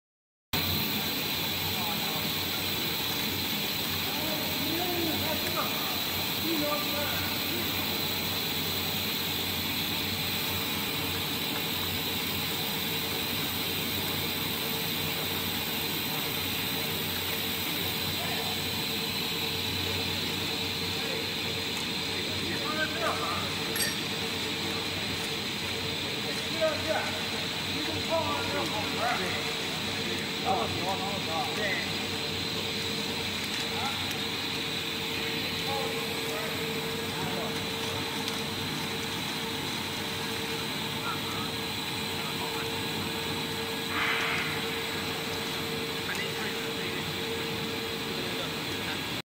Plastic melting extruder
Extruder testing for plastic pallet production.